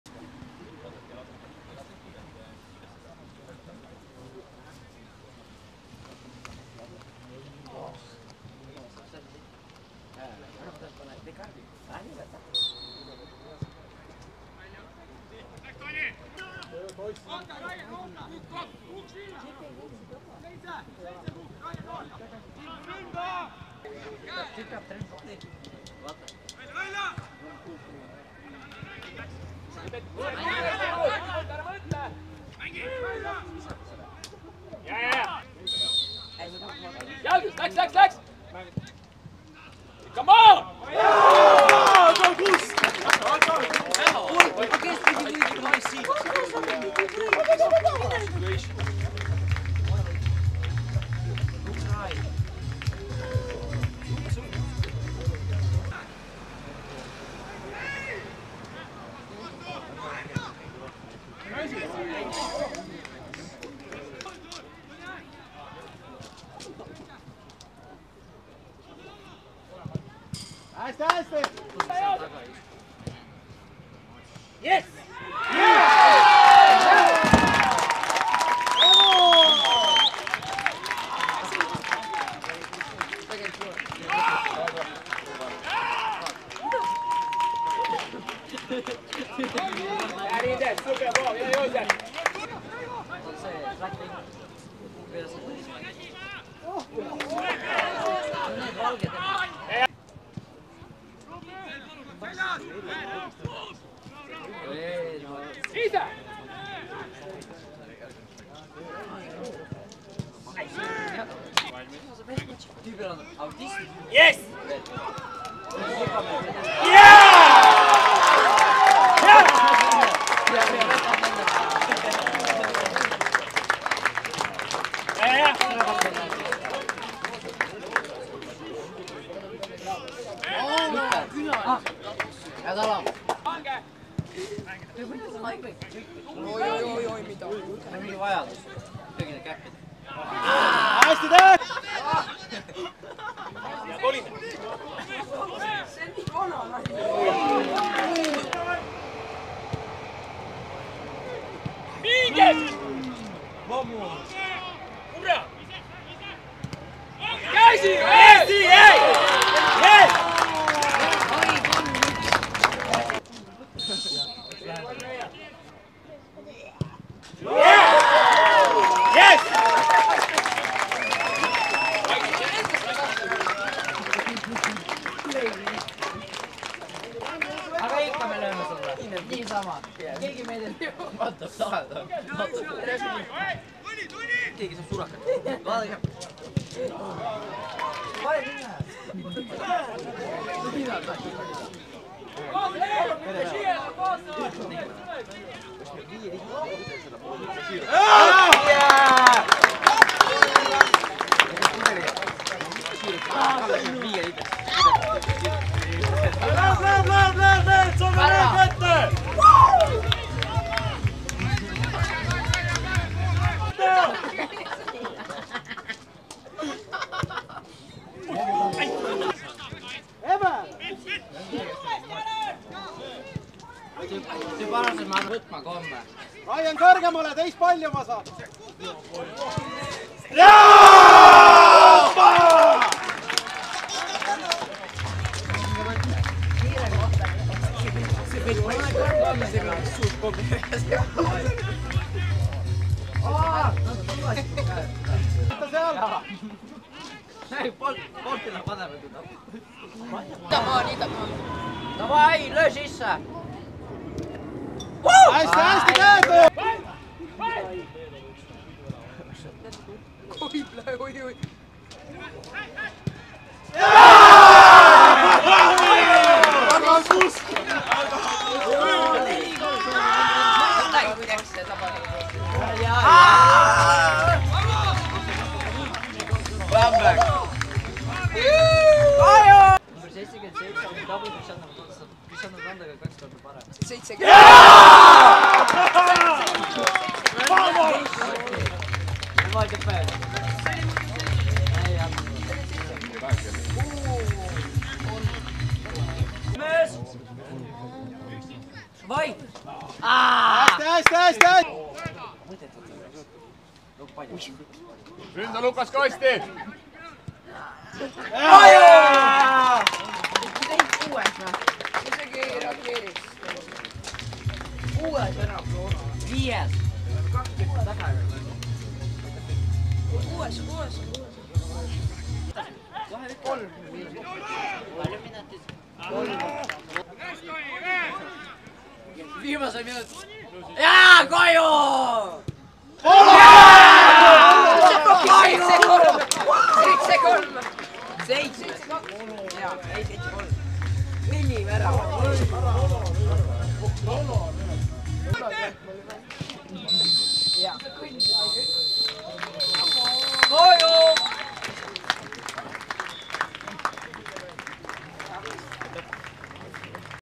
Tallinna FC Flora U21 - JK Tabasalu | 7:3 | Esiliiga 24. voor | 2023
21. augustil kell 18.45 Lilleküla harjutusväljakul

Tallinna FC Flora U21 - JK Tabasalu 7:3 (1:1)
21. Oscar Pihela, 62.,69. Tony Varjund, 74.,89. Bruno Vain, 81. Roland Lukas, 90+1. Anton Volossatov - 38., 59. pen. Joosep Põder, 50. Peeter Klein